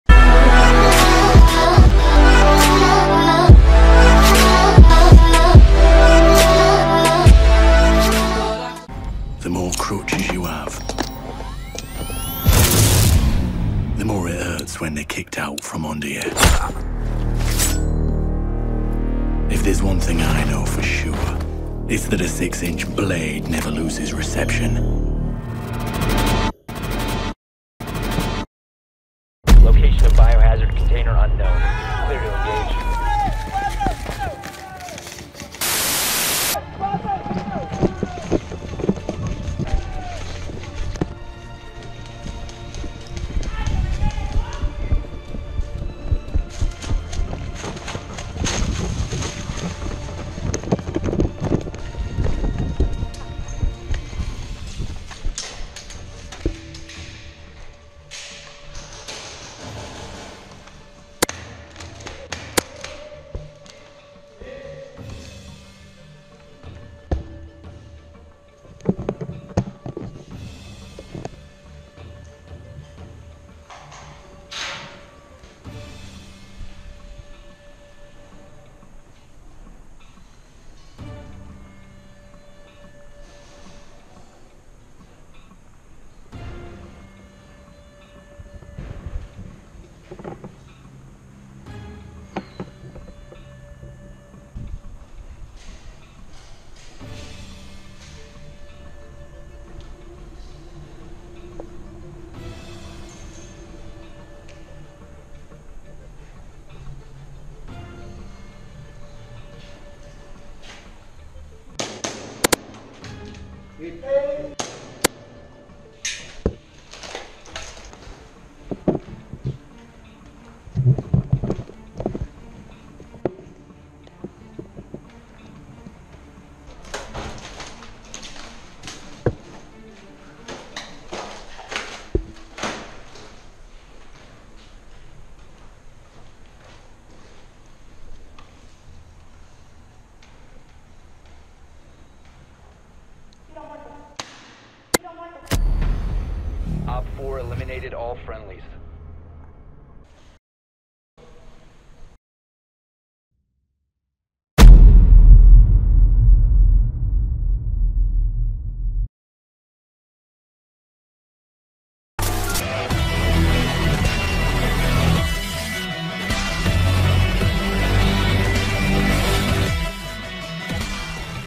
Rainbow Six Siege Operator Thatcher - Airsoft Cosplay - CYMA M870 Shotgun Gameplay
In this video I’m cosplaying as one of my favorite Operators in the game Rainbow Six Siege, using the CYMA M870 3 Round Airsoft Shotgun. Remember to comment if you want me to do more R6 cosplay airsoft gameplays.